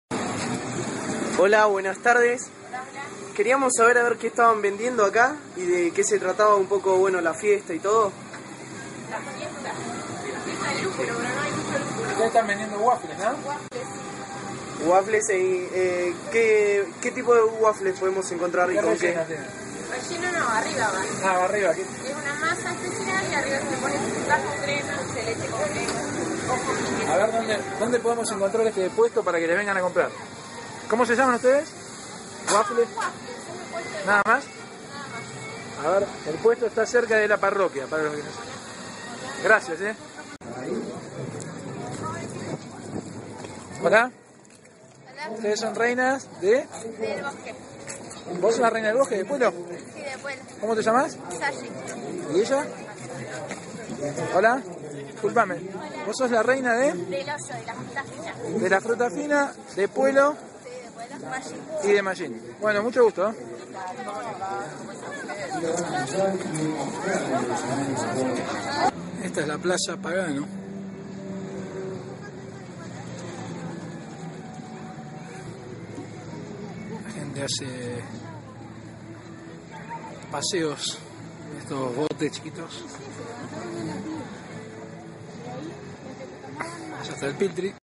Fiesta Nacional del Lupulo - El Bolsón
Recorriendo el predio de la Fiesta Nacional del Lupulo en El Bolsón Patagonia Argentina. Algunas de las reinas de las fiestas regionales, wafles y Plaza Pagano.